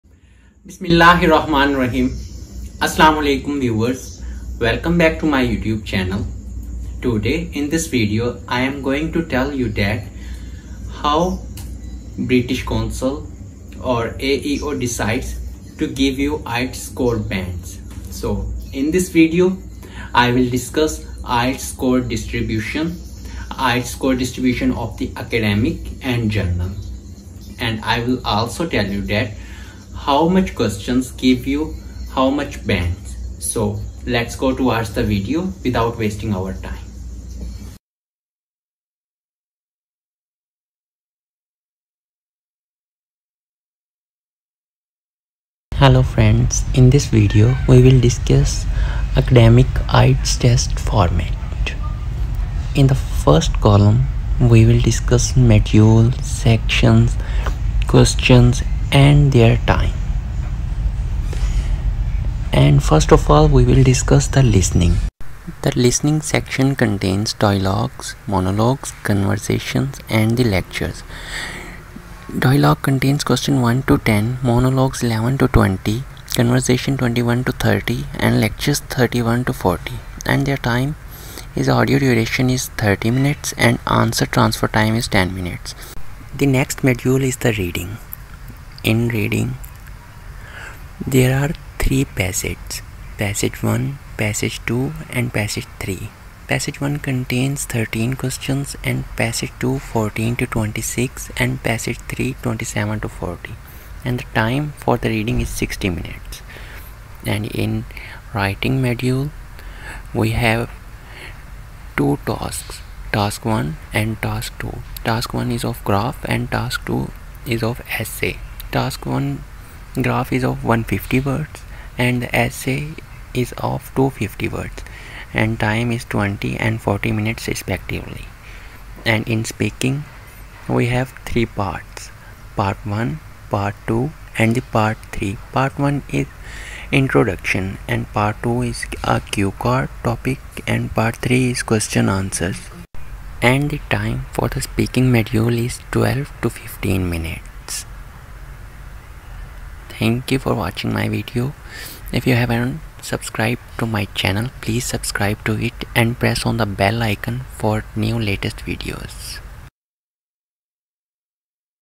IELTS ACADEMIC TEST FORMAT | IELTS LATEST TEST PATTERN
IELTS Speaking test: 10 tips from Experts
Tip 1: Don't memorise answers.
Tip 2: Don't use big and unfamiliar words.
Tip 3: Use a range of grammatical structures.
Tip 4: Don't worry about your accent.
Tip 5: Pause to think.
Tip 6: Avoid using fillers.
Tip 7: Extend your answers.
Tip 8: Smiling helps pronunciation.

to get the PDF of IELTS Books 16